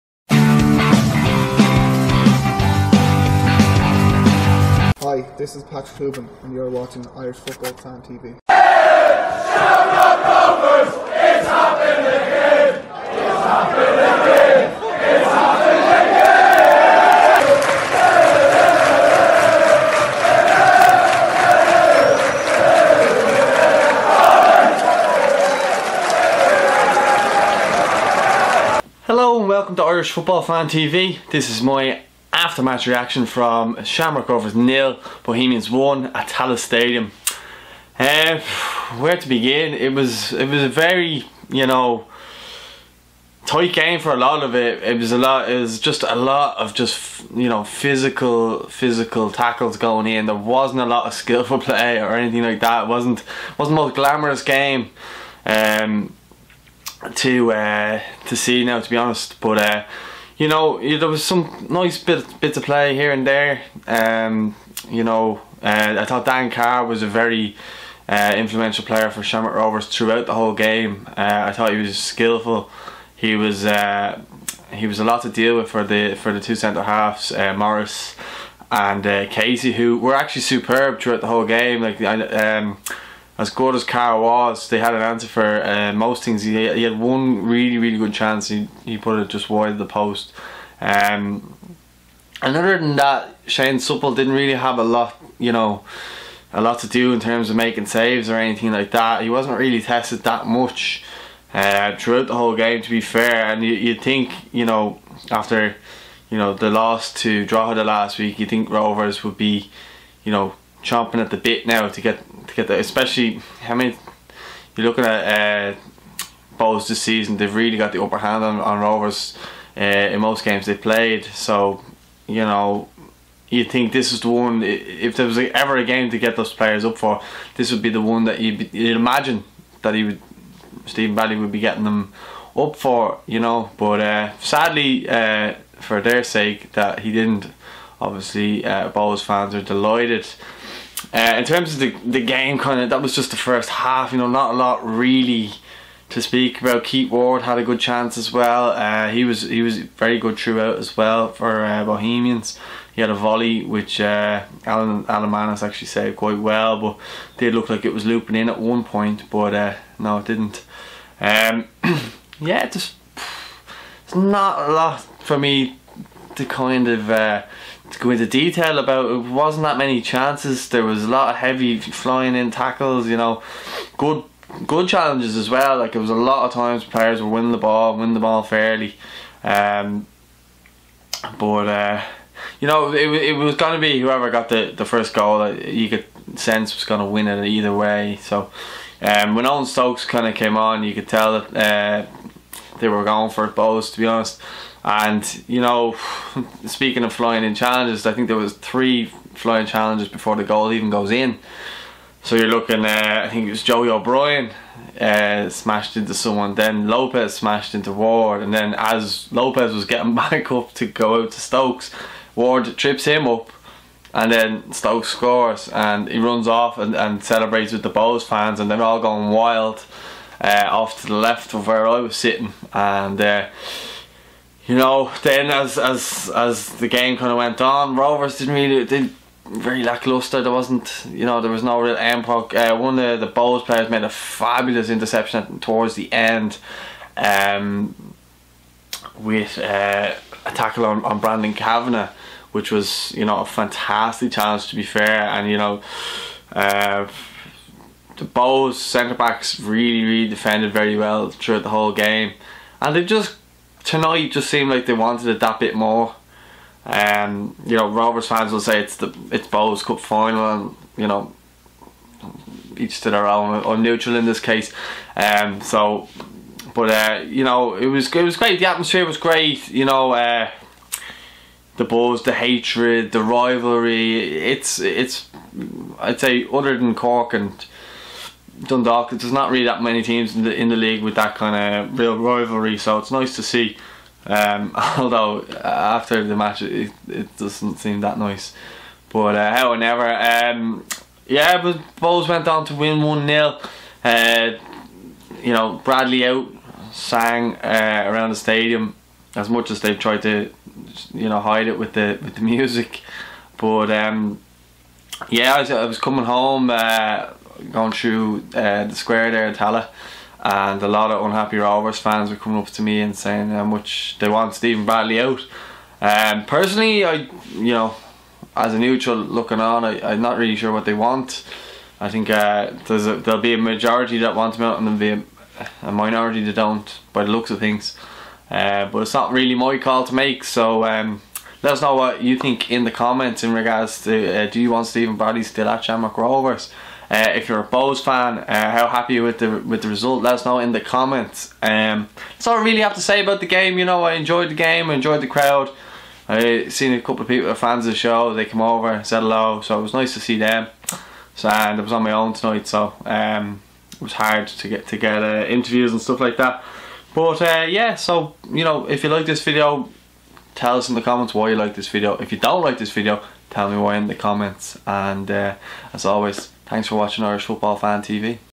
Shamrock Rovers 0-1 Bohemians | Stokes wins it as "Bradley Out" rings around Tallaght Stadium
Graphics By: @Aaron Howey